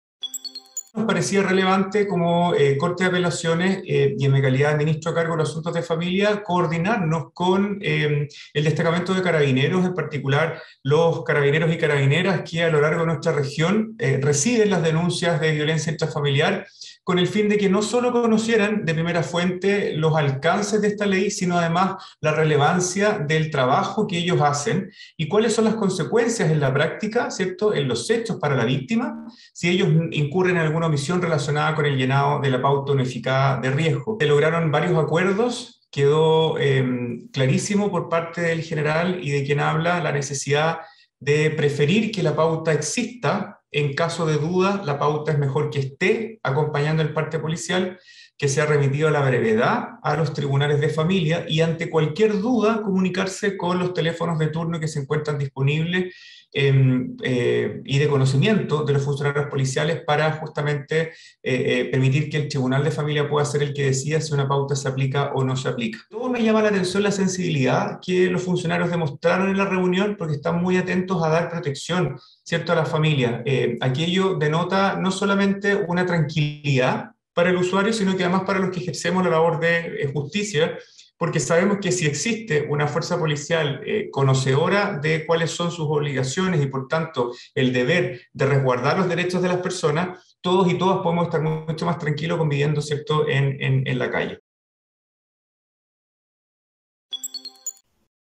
Corte de La Serena acuerda trabajo colaborativo con Carabineros en casos de víctimas de VIF
El ministro de la Corte de Apelaciones de La Serena, Felipe Pulgar Bravo, encargado de Asuntos de Familia, se reunió vía zoom con carabineros de las distintas unidades policiales de la región de Coquimbo con el fin de transmitir los principales aspectos de la Pauta Unificada de Evaluación de Riesgo, PUIR.